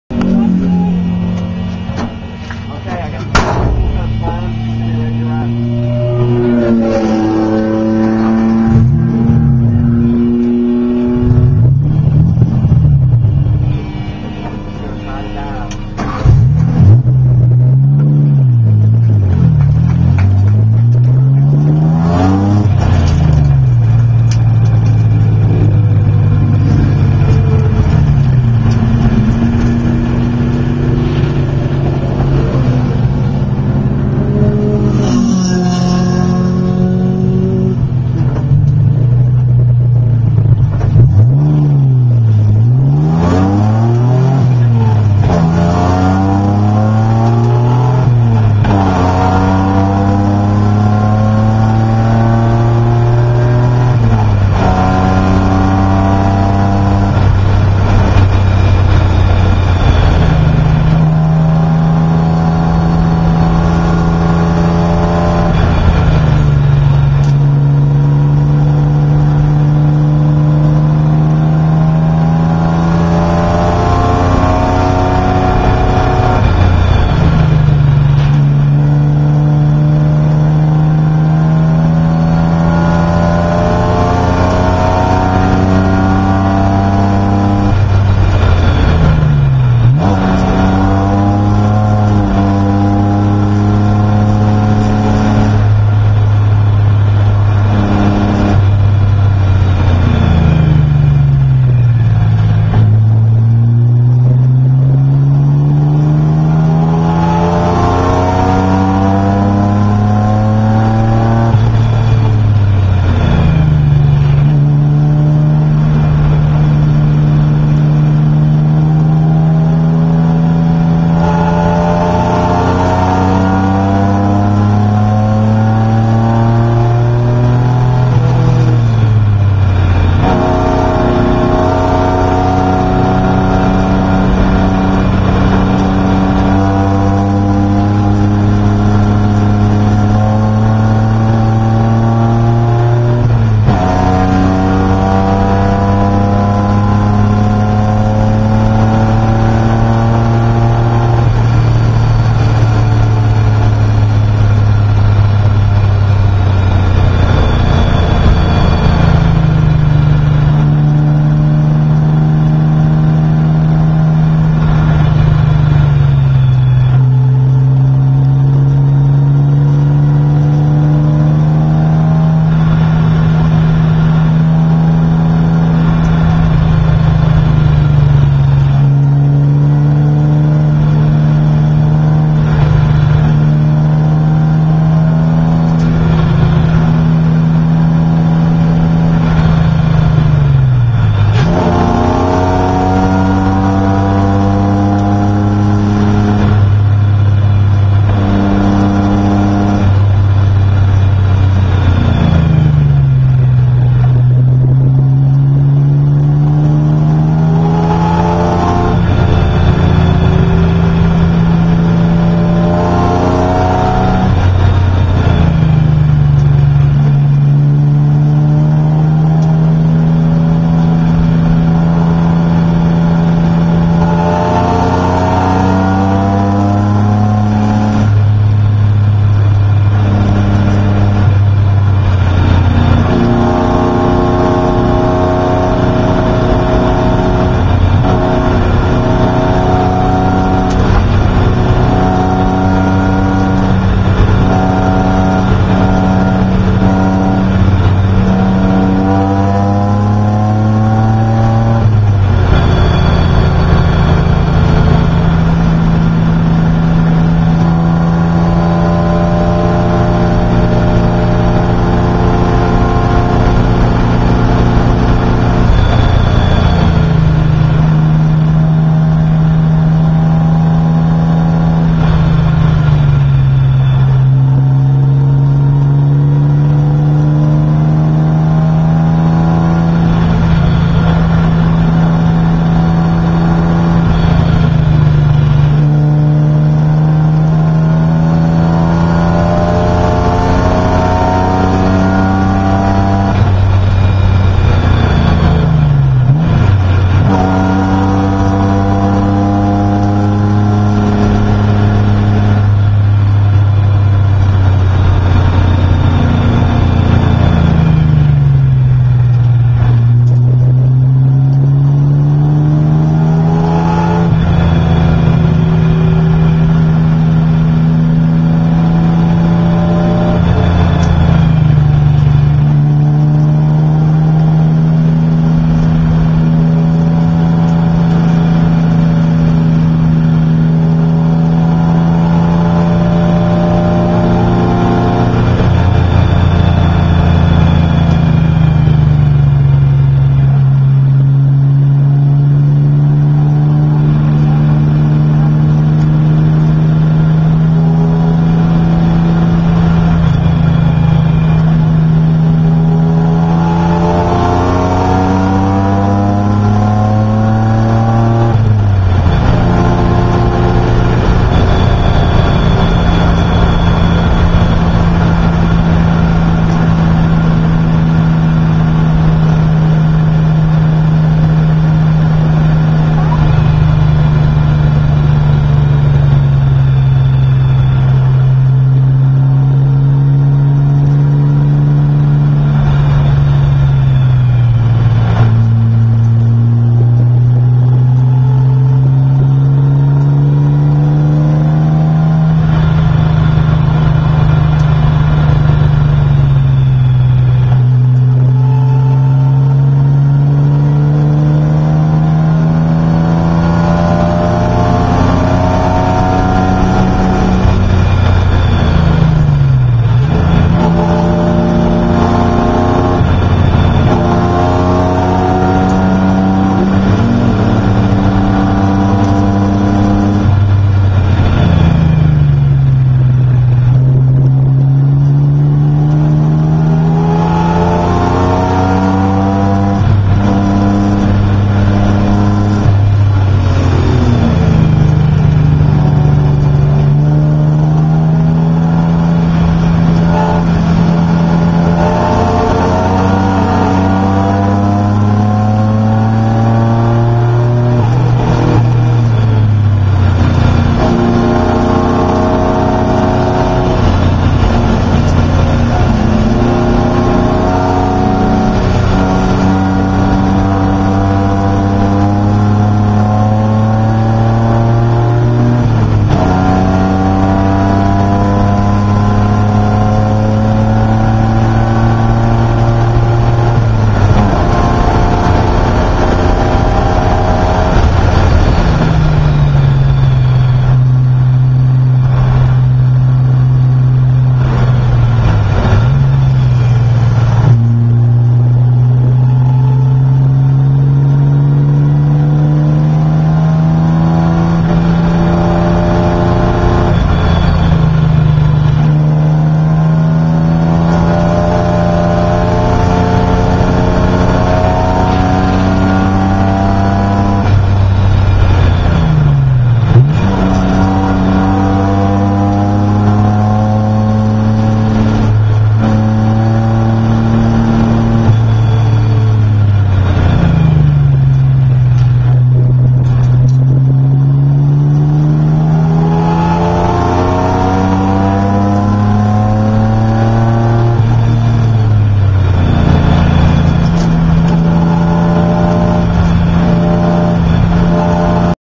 Chump Car Putnam Park
Just a few minutes of Julie starting out before the camera went dead